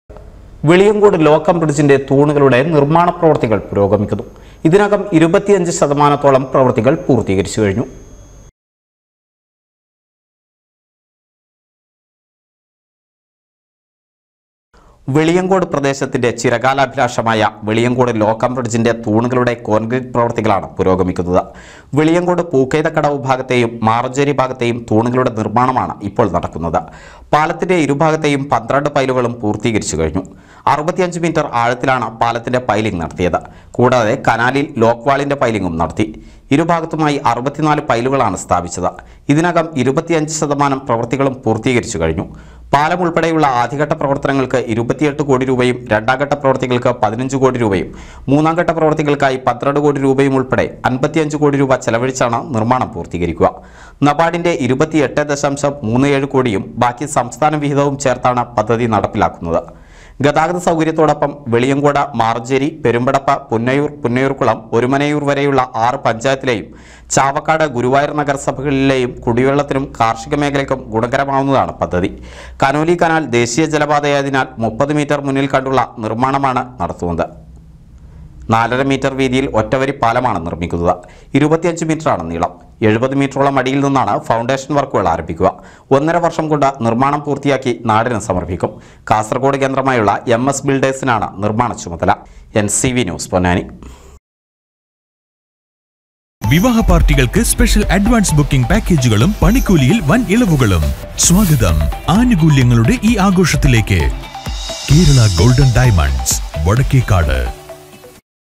വെളിയങ്കോട് ലോക് കം ബ്രിഡ്ജിന്‍റെ തൂണുകളുടെ നിര്‍മ്മാണ പ്രവൃത്തികള്‍ പുരോഗമിക്കുന്നു
വെളിയങ്കോട് ലോക് കം ബ്രിഡ്ജിന്‍റെ തൂണുകളുടെ നിര്‍മ്മാണ പ്രവൃത്തികള്‍ പുരോഗമിക്കുന്നു. ഇതിനകം 25 ശതമാനത്തോളം പ്രവൃത്തികളാണ് പൂര്‍ത്തിയായത്